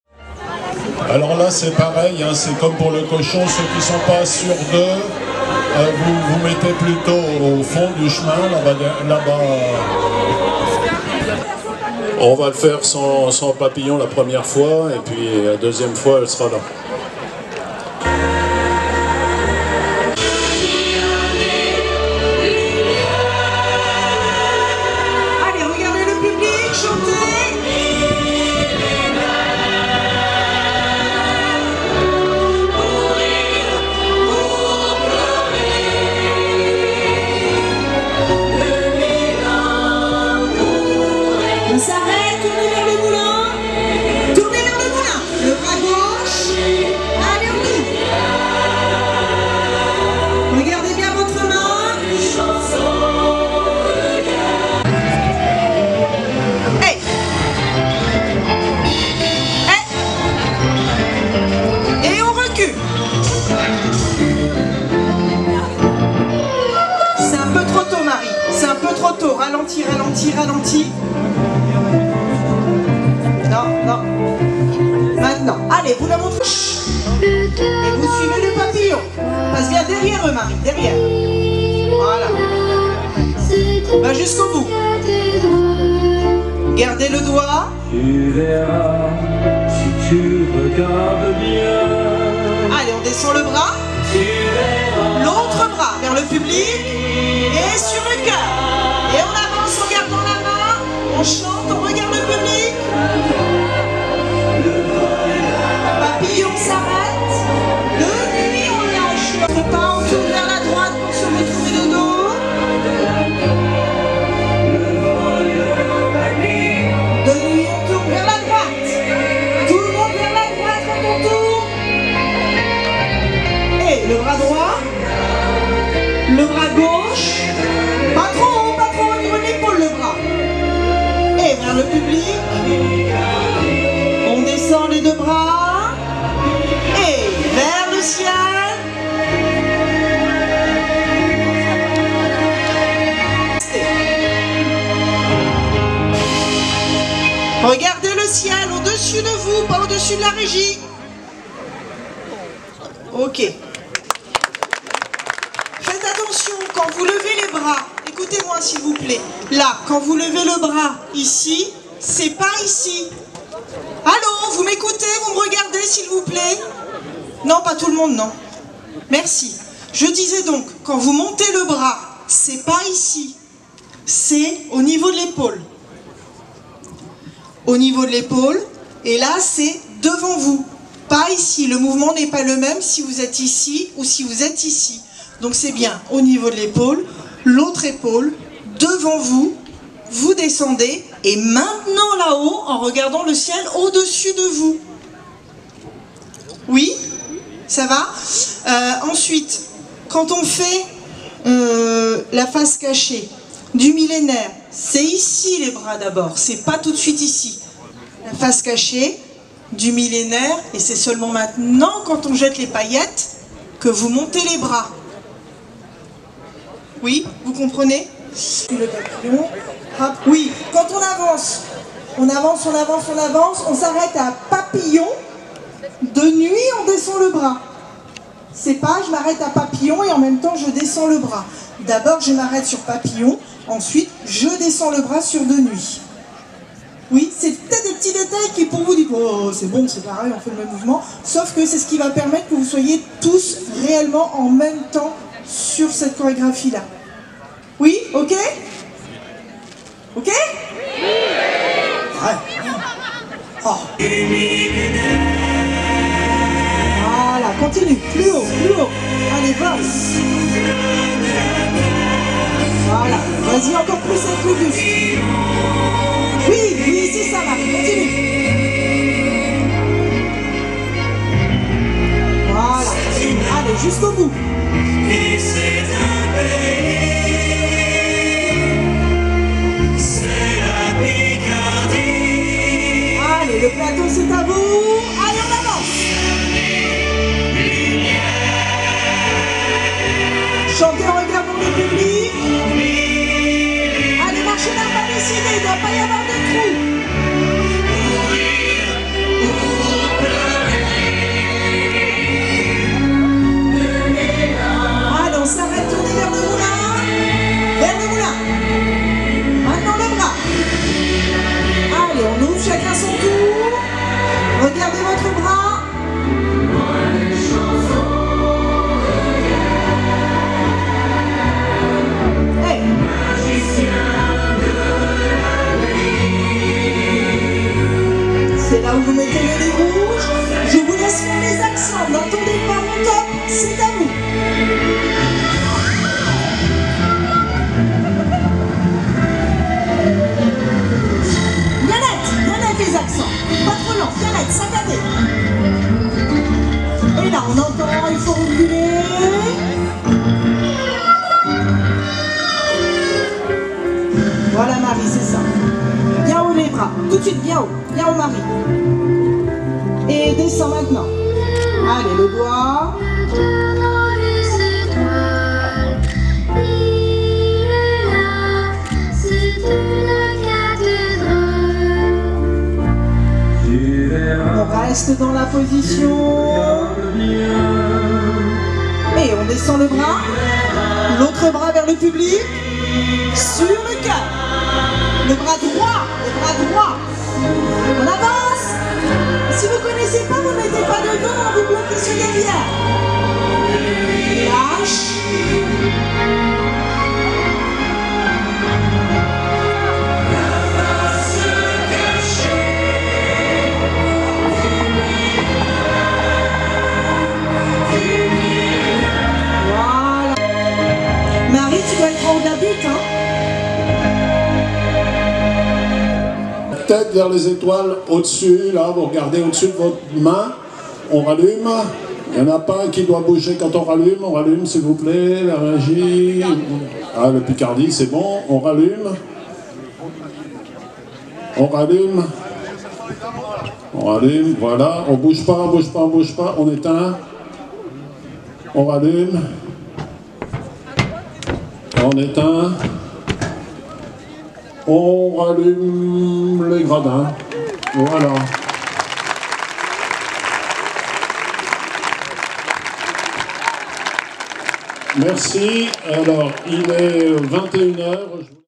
Somme : c'était la dernière répétition générale avec le grand lancement du Souffle de la Terre 2023
Dans la soirée du jeudi 24 août, c'était la grande répétition nocturne du Souffle de la Terre, à Ailly-sur-Noye, avant la première représentation de l'édition de grand spectacle son et lumière, ce vendredi 25.

Depuis le gradin, le metteur en scène Dominique Martens et de la chorégraphe Barbara Hoguet ont guidé les bénévoles, pour les derniers ajustements, donnant des recommandations, guidant les comédiens pour obtenir les déplacements et la gestuelle les plus justes.